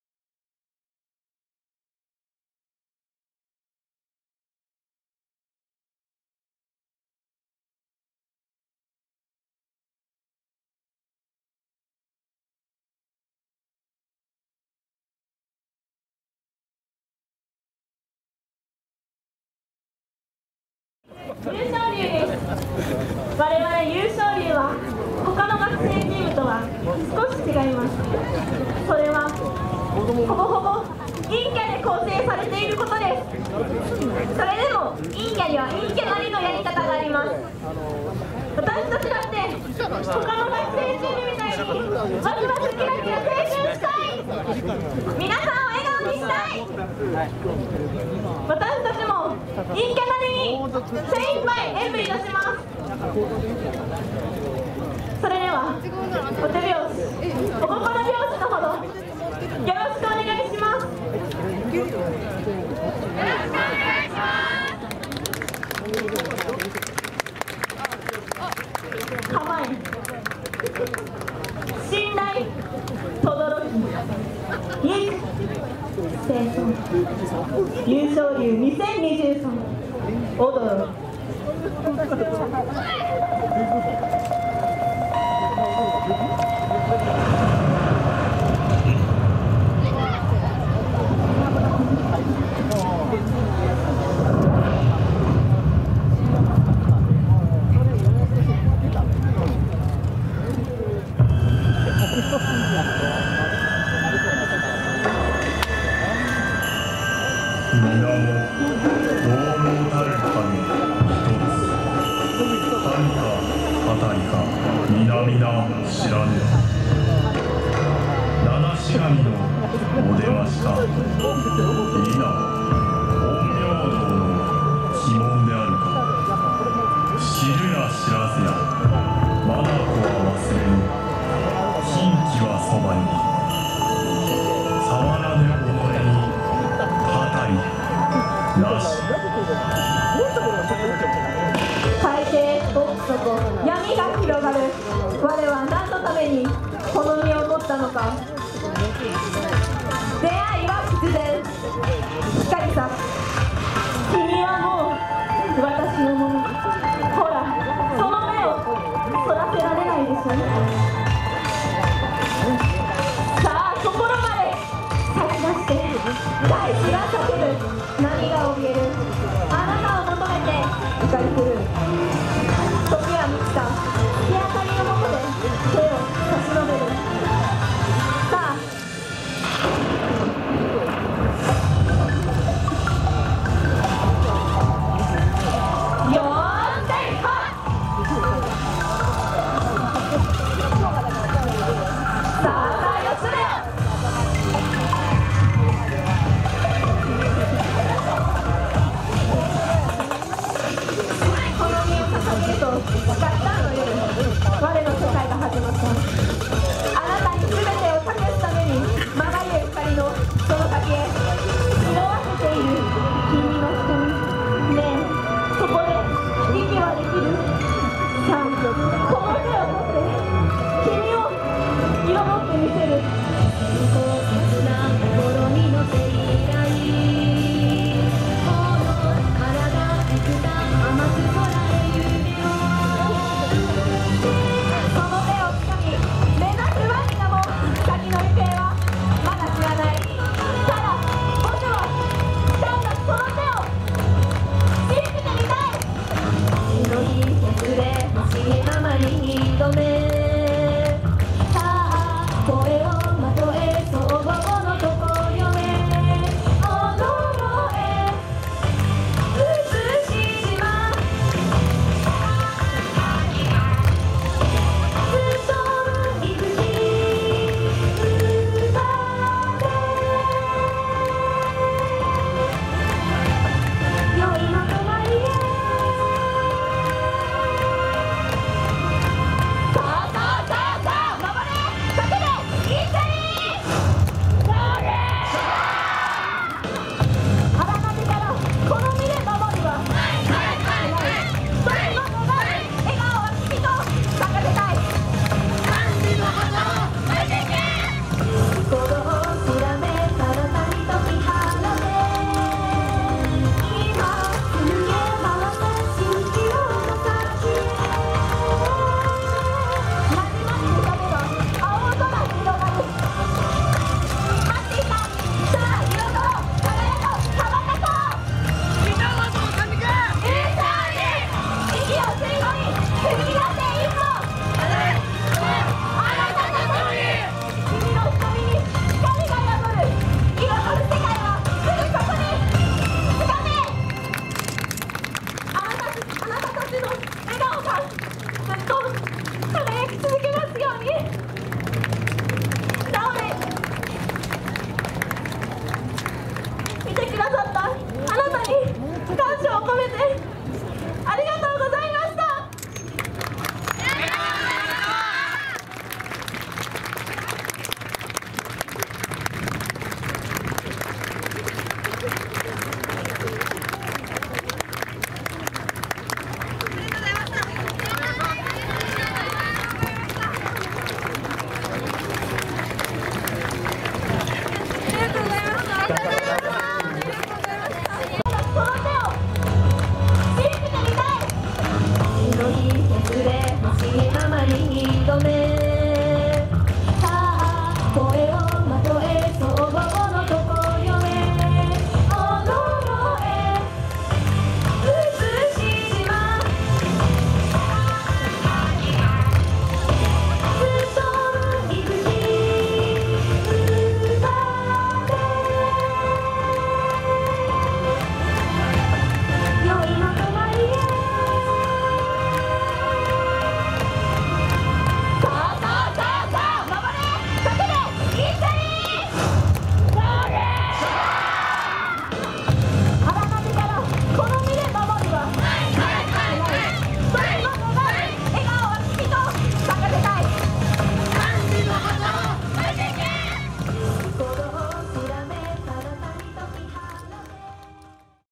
20231022横浜よさこい祭り2023（北里大学よさこいチーム北里三陸湧昇龍）赤レンガ防波堤会場【ＨＤ・原画４Ｋ】
横浜よさこい祭り2023（北里大学よさこいチーム北里三陸湧昇龍）赤レンガ防波堤会場【ＨＤ・原画４Ｋ】
「横浜国際仮装行列写真館」に写真（1600×1067と800x534の
２サイズ）をアップしています